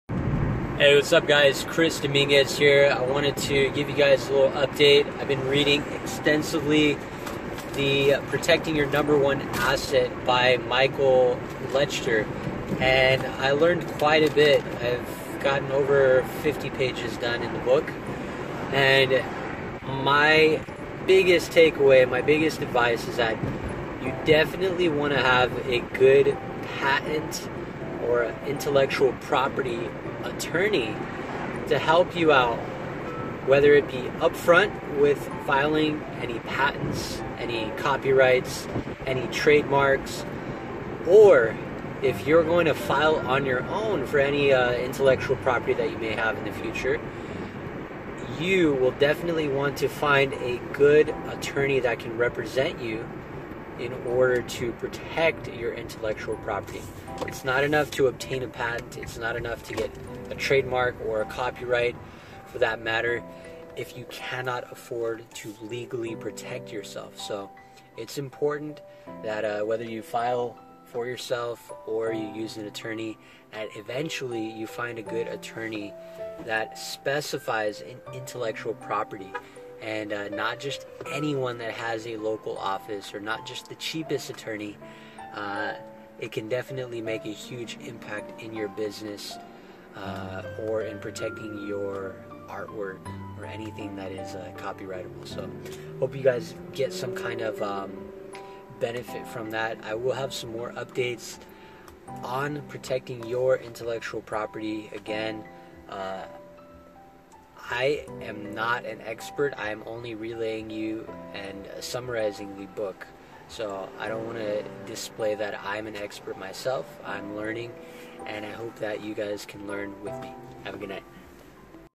Book Review for Protecting Your #1 Asset by Michael Lechter: Be Willing to get Legal Assistance
Although it is entirely possible for you to file and even represent yourself when it comes to your intellectual property, it is highly recommended by myself and experts to get legal advice from an intellectual property attorney. Intellectual property is a important part of any businesses planning and long-term development and is well worth the money invested.